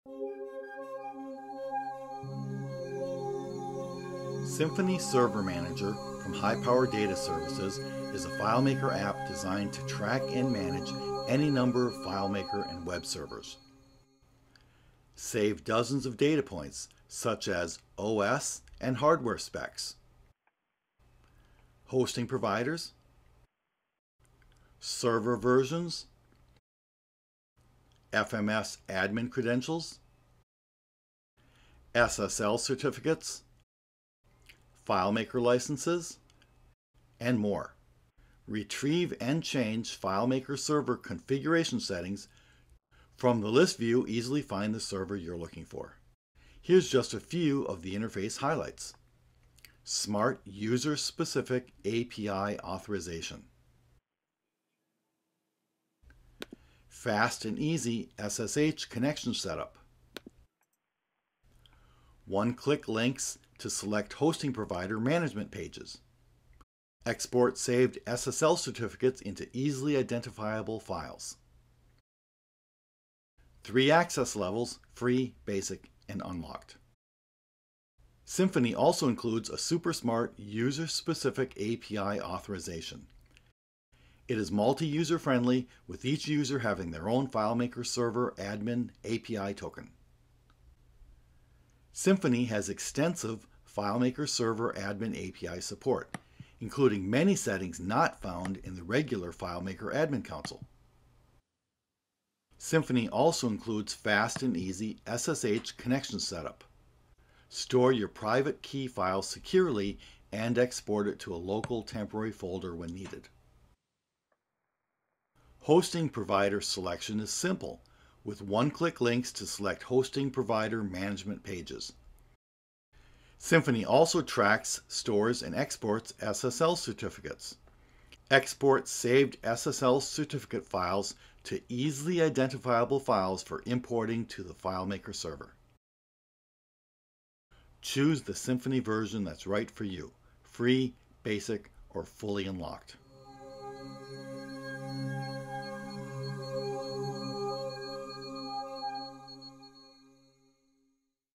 SymphonyServer Intro
Intro to Symphony Server tool for tracking FileMaker Admin Console logins, SSL certificates, FileMaker Licenses, SSH Remote login keys, and more